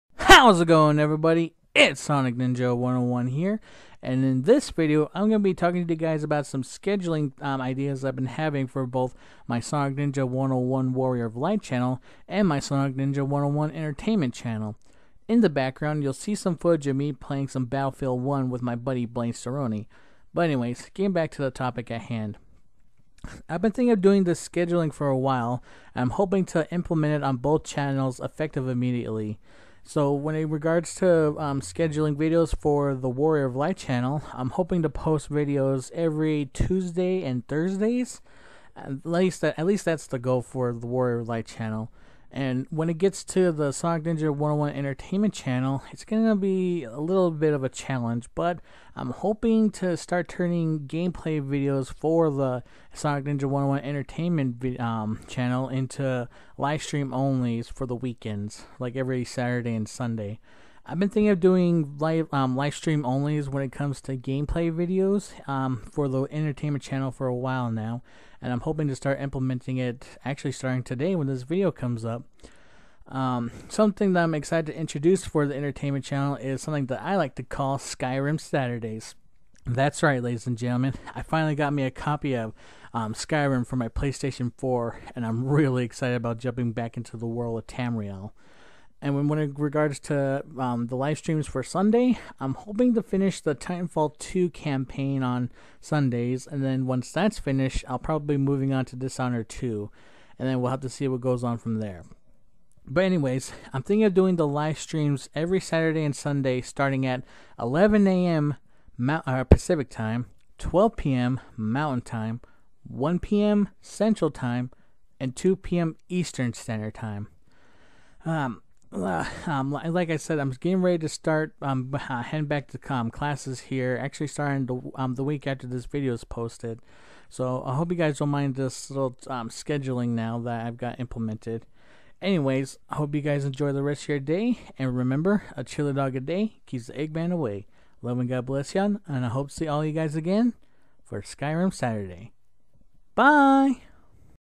My New Schedule
Hope You guys don't mind my new scheduling for the channels